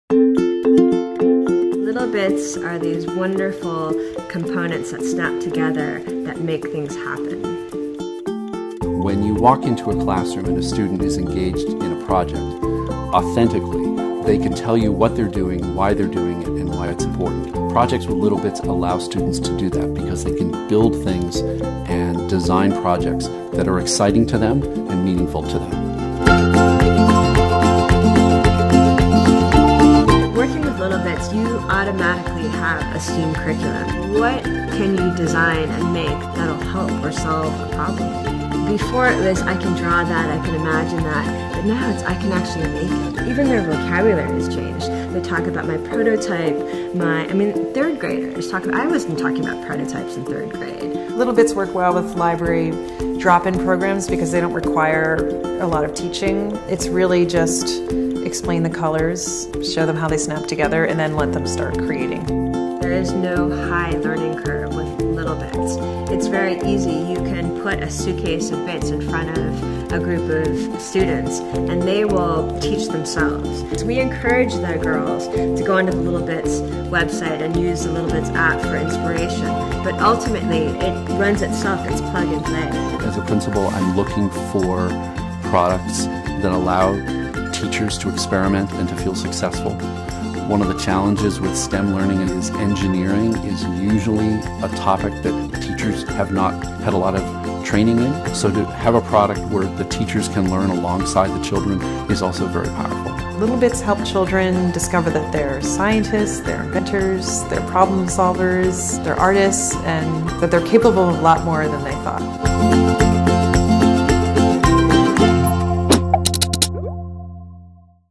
littleBits Steam Student Set
Easy to teach. Fun to use. The littleBits STEAM Student Set is the easiest way to integrate powerful STEM/STEAM learning into the classroom. Designed with educators, the toolbox engages students in learning through creating inventions while linking to NGSS/Common Core standards.